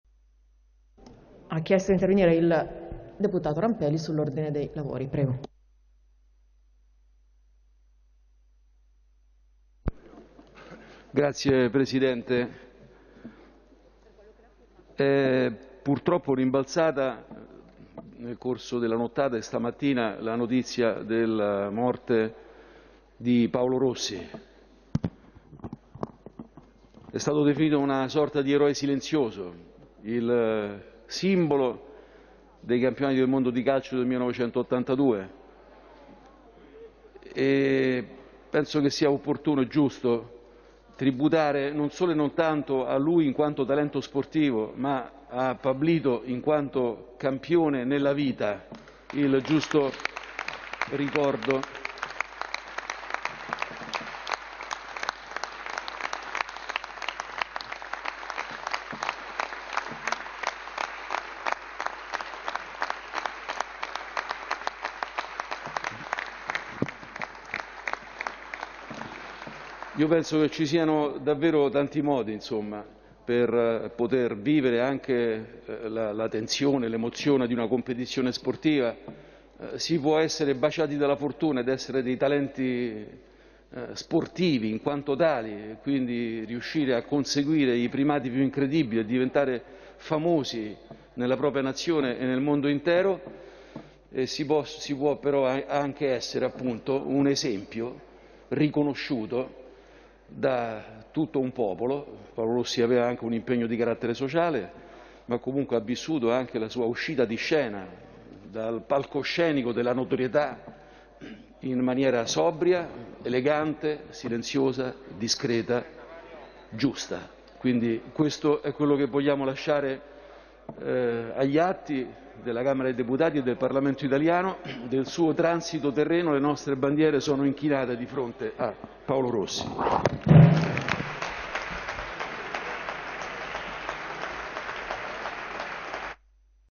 Paolo Rossi, Rampelli lo ricorda in Aula alla Camera
\"E'' stato innanzitutto un campione nella vita\", così Fabio Rampelli (FdI) Vicepresidente della Camera dei deputati

Roma, 10 dicembre 2020
\"È rimbalzata la notizia della morte di Paolo Rossi, definito eroe silenzioso, simbolo della vittoria azzurra nei Campionati del mondo del 1982.
Sentiamo il dovere di ricordare il suo talento sportivo ma ‘Pablito', così definito per la leggendaria tripletta contro il Brasile, è stato innanzitutto un campione nella vita. Ci sono tanti modi per vivere la competizione sportiva. Si possono conquistare primati su primati, diventare per questo famosi in tutto il mondo, strafare, vivere sopra le righe oppure si può essere esempio riconosciuto di tutto un popolo. Paolo Rossi ha vissuto la sua uscita di scena in modo sobrio ed elegante. Davanti  al suo transito terreno le nostre bandiere s'inchinano\".
È quanto ha dichiarato il vicepresidente della Camera  Fabio Rampelli intervenendo in aula dai banchi di Fratelli d'Italia in apertura di seduta. / Camera web ( Agenzia Vista/Alexander Jakhnagiev - CorriereTv ). Guarda il video su Corriere